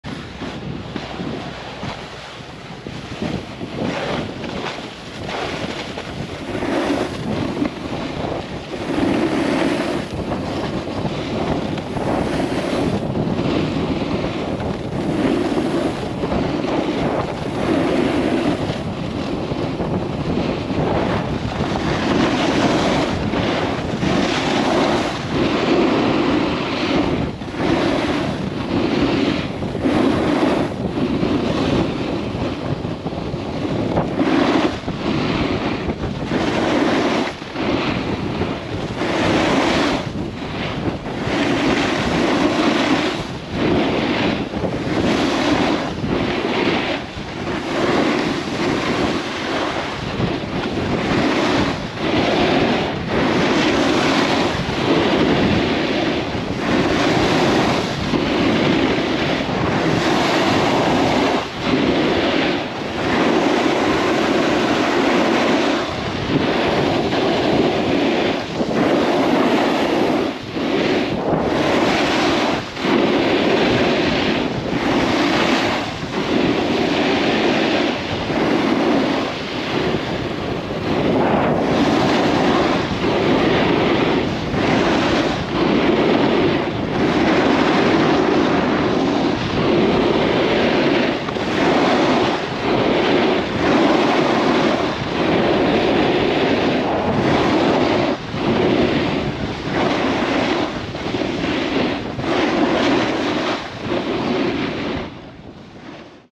Gravy | Arizona Snowbowl Nov 27, 2024
A grey and foggy day at the bowl.